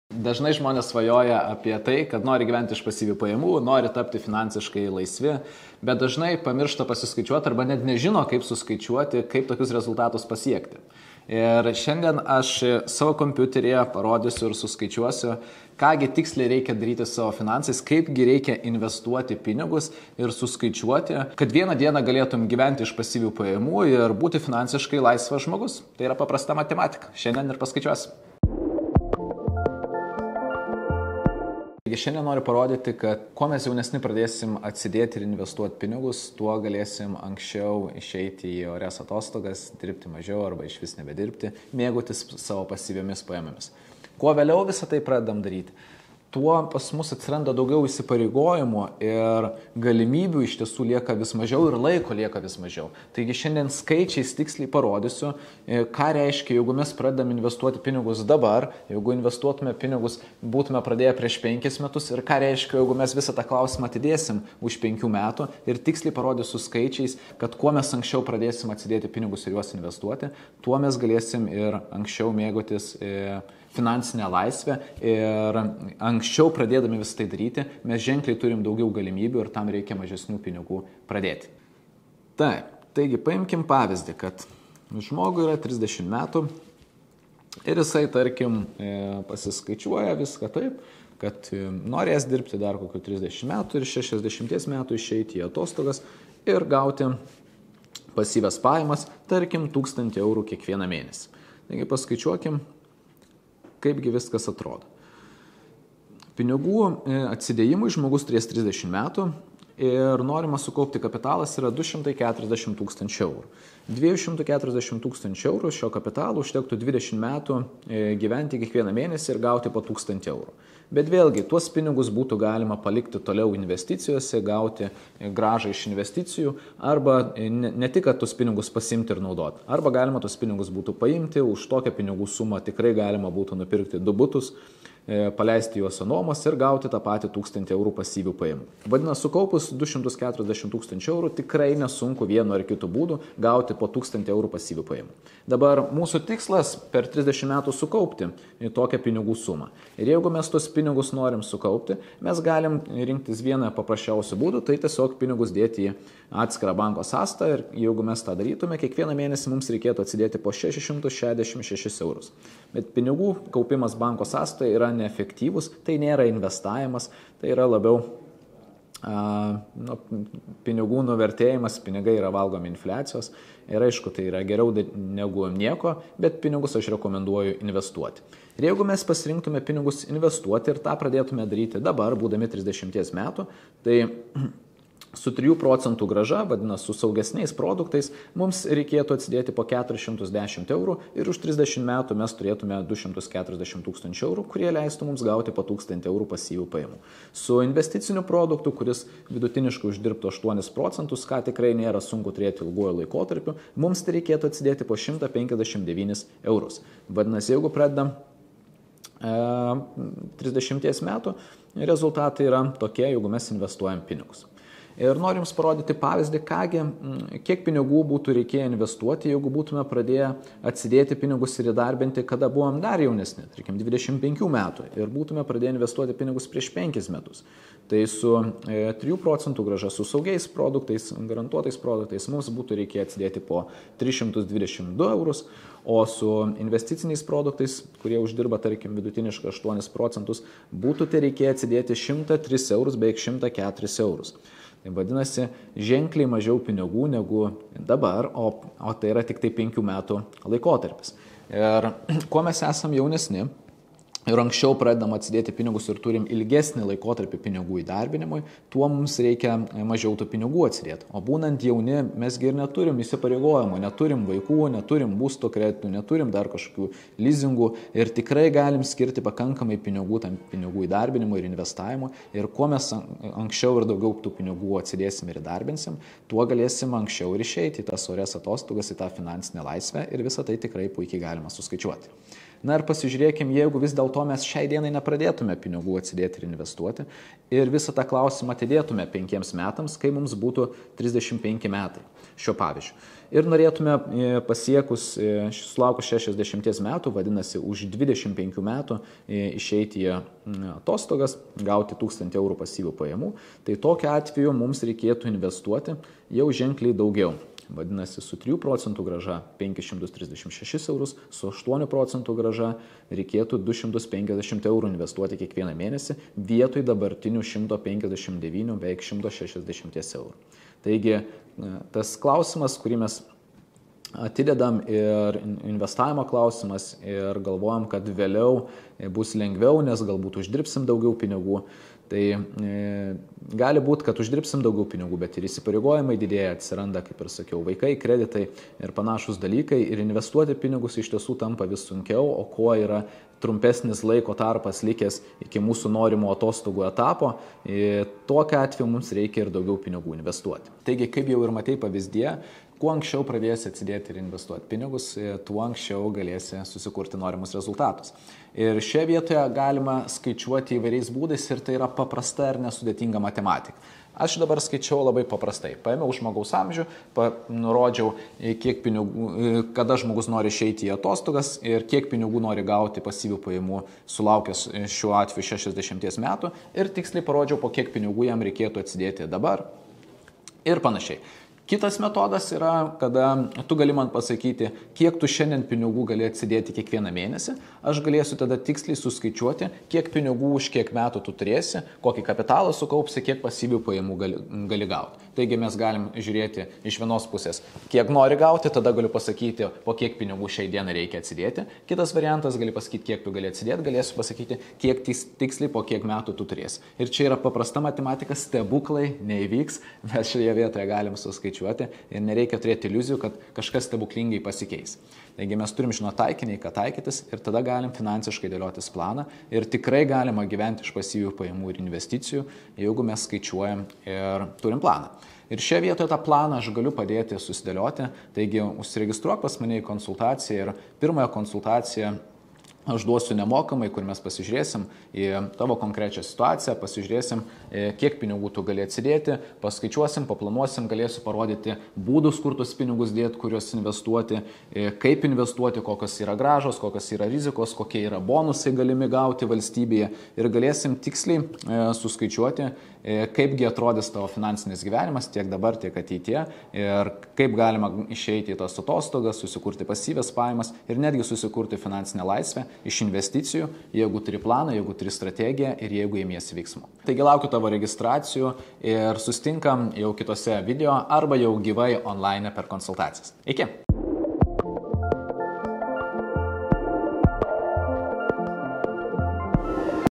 Kaip suskaičiuoti kada galėsi gyventi iš pasyvių pajamų ir būsi finansiškai laisvas ?
Jeigu įdomu turėti suvokimą ir asmeninį planą, kaip pasiekti savo finansinius tikslus registruokis į nemokamą ONLINE konsultaciją.

Visi svajojam apie pasyvias pajamas ir finansinę laisvę, bet retas žinome kaip tai pasiekti. Šiame video nepateikiu kažkokių super duper paslapčių kaip susikurti magišką pasyvių pajamų šaltinį. Šiame video parodau realų skaičiavimo pavyzdį kiek reikėtų investuoti kiekvieną mėnesį norint pasiekti savo finansinį tikslą, kuris generuotu pasyvias pajamas ateityje.